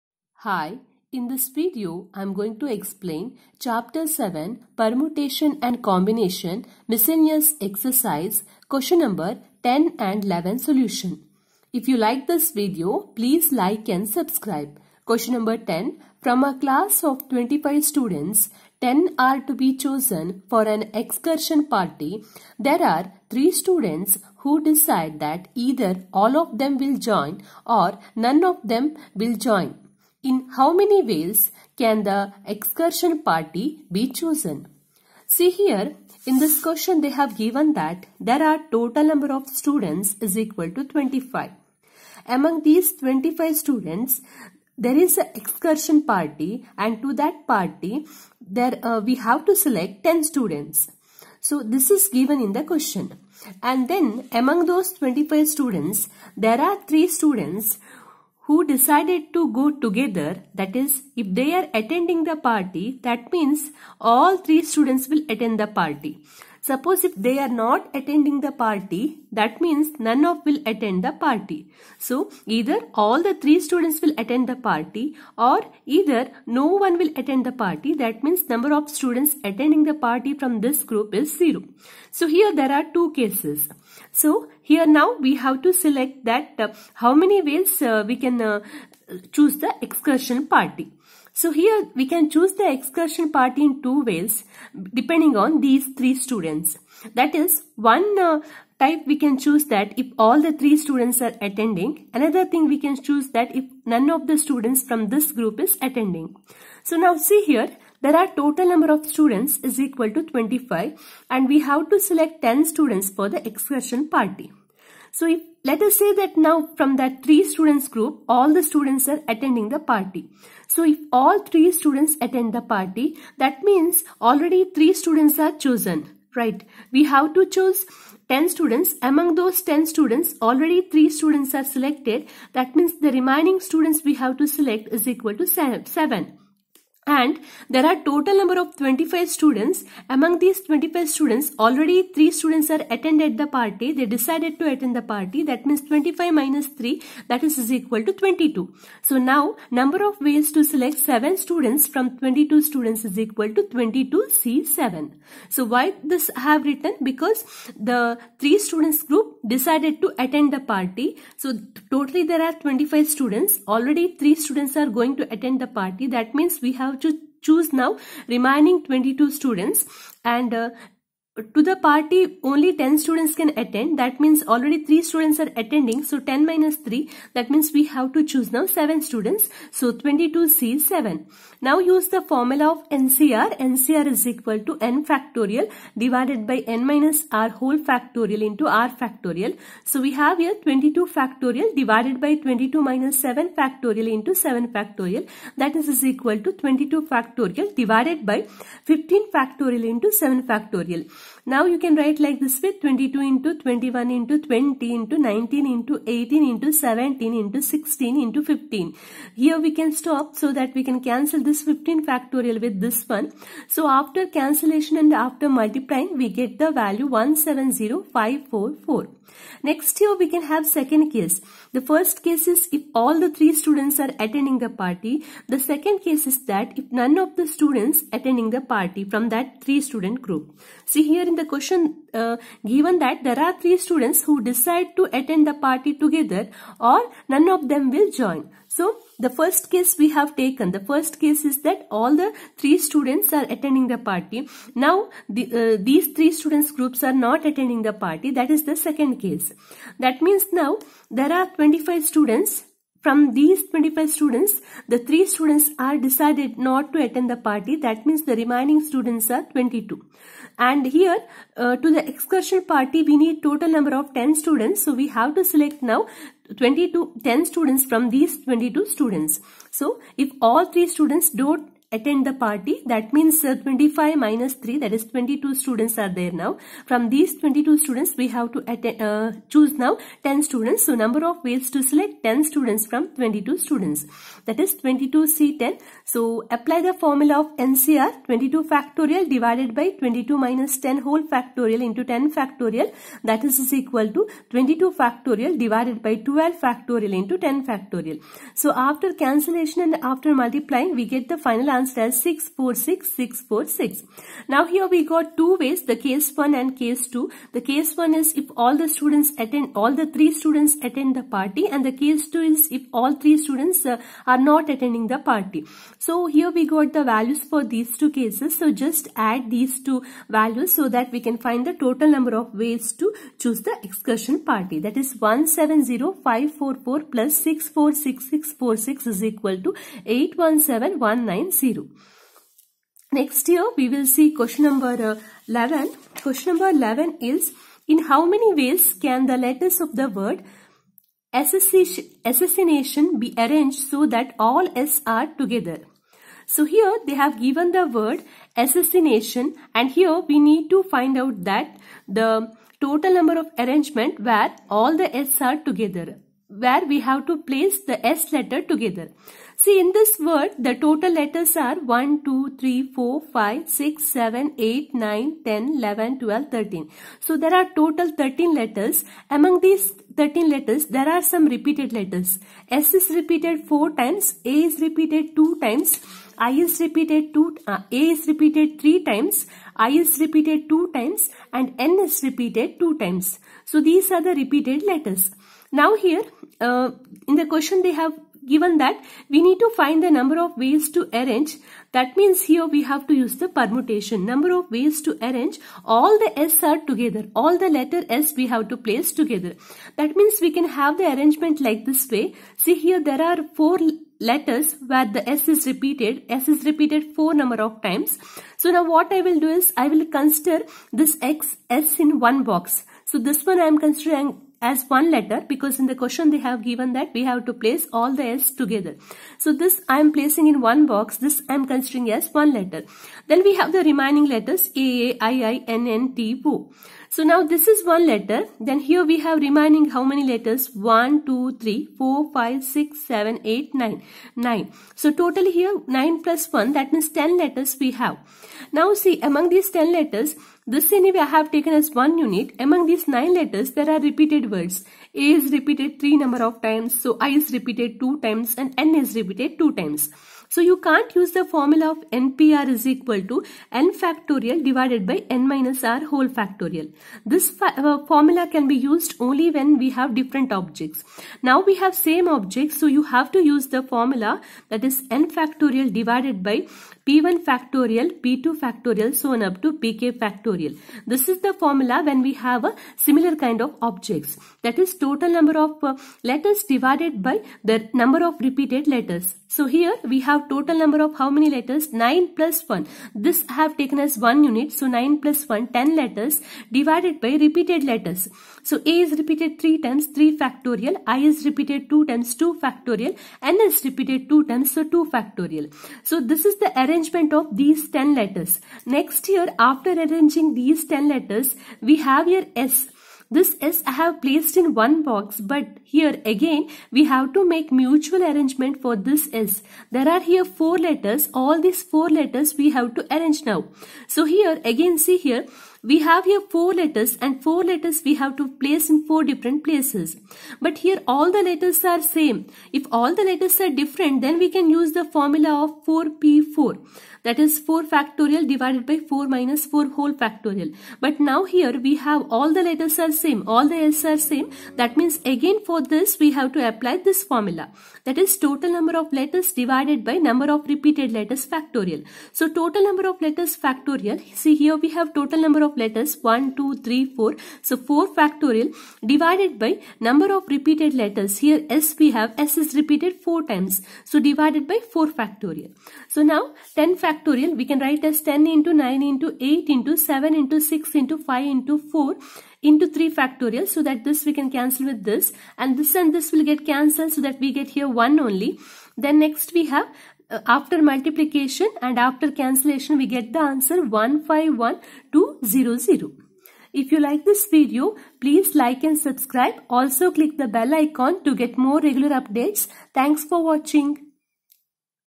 18. NCERT Maths Class 11: Chapter 7 - Permutations and Combinations: Solution of Misc. Ex. Q10-Q11
Hello friends, I will be covering NCERT class 11 mathematics in this series of uploads on my channel. In this series theory of the concept will be followed by explanation of the solution. Hope to help all the students to understand the concept of mathematics and also to do well in the examinations.